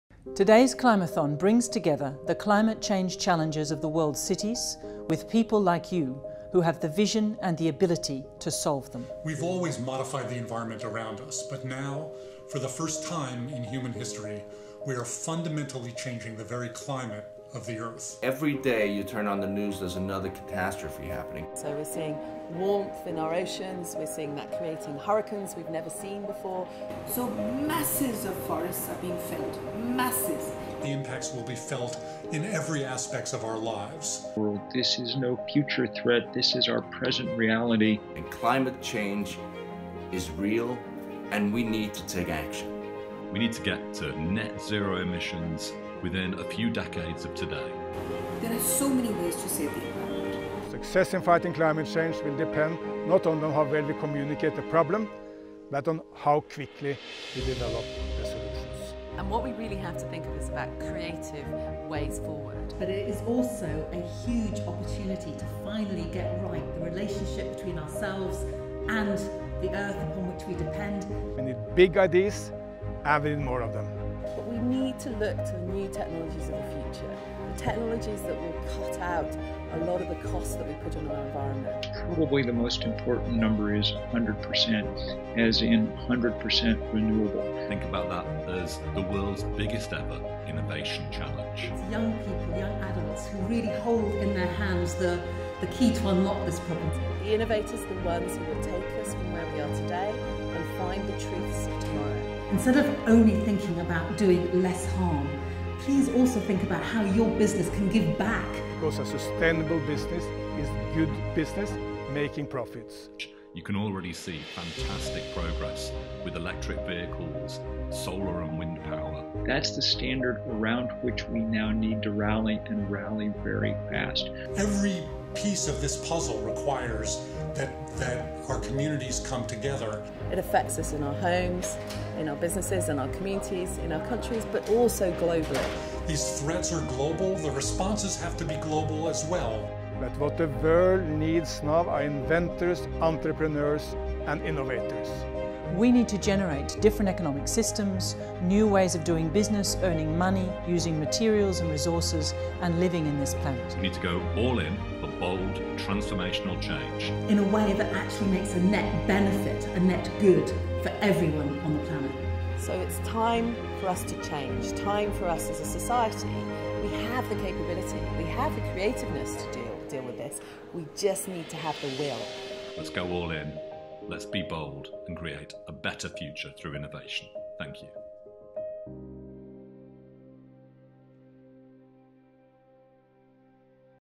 Welcome to Climathon 2017
Climathon is a global 24-hour climate action hackathon which will take place simultaneously in major cities around the world on 27 October 2017.

It brings together the challenges of the world’s cities with the people who have the passion and ability to solve them. Climathon attracts innovators, entrepreneurs, students and professionals to create innovative solutions to cities climate challenges.